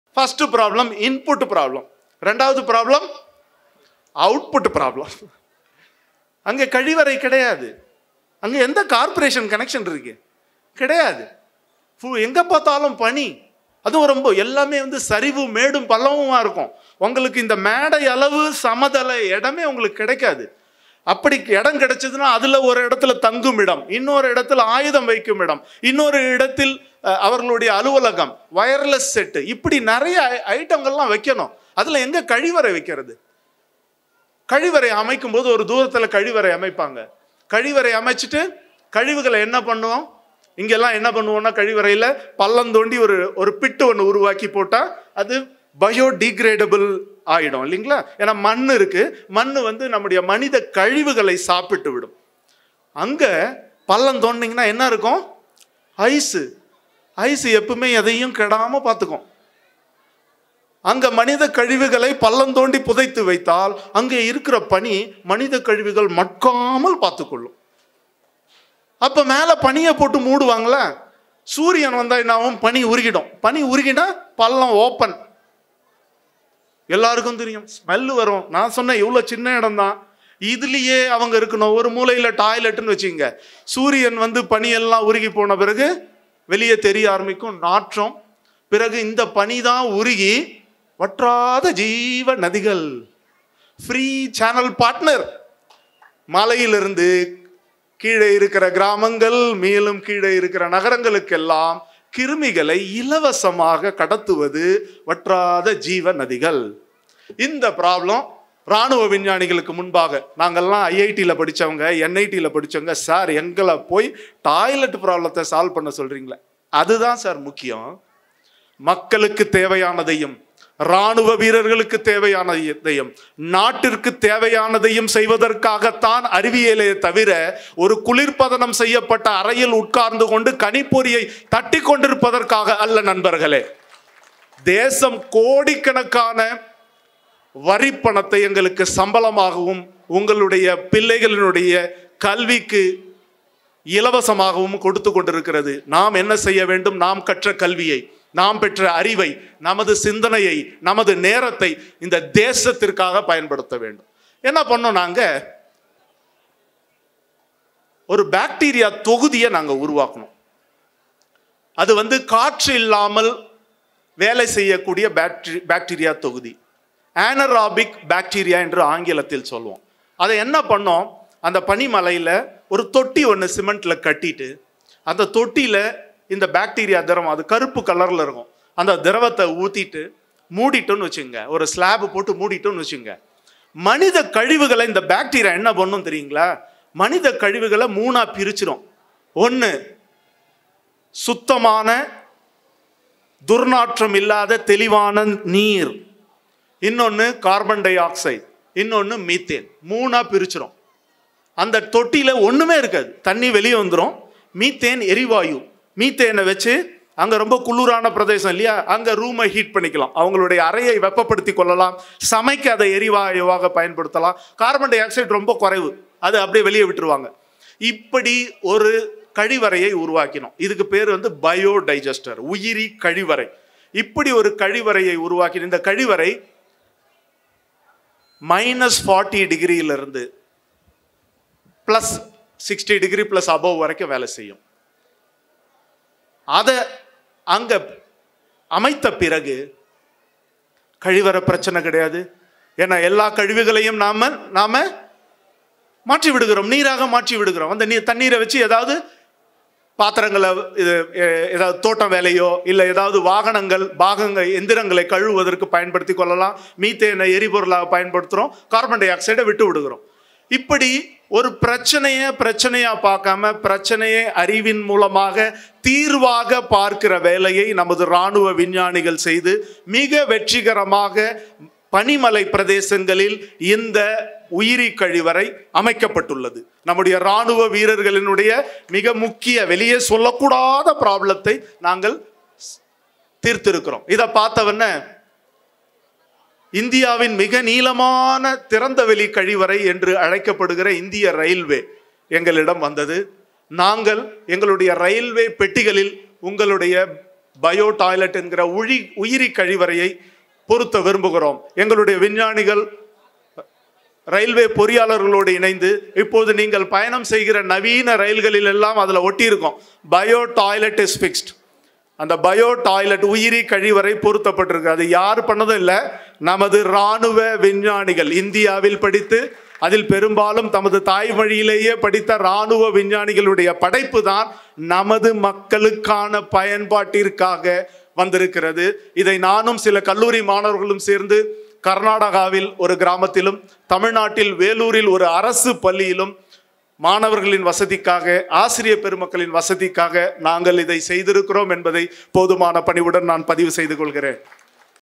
இராணுவ வீரர்களின் முக்கியமானபிரச்சனையை தீர்த்தோம் | V Dillibabu Speech
ஓசூரில் நடைபெற்ற புத்தகத்திருவிழாவில் பாதுகாப்பு ஆராய்ச்சி மற்றும் மேம்பாட்டு துறை, முதுநிலை விஞ்ஞானி வி.டில்லிபாபு ” இராணுவ ஆராய்ச்சியின் சுவாரசிய பக்கங்கள் என்ற தலைப்பில் உரையாற்றினார்.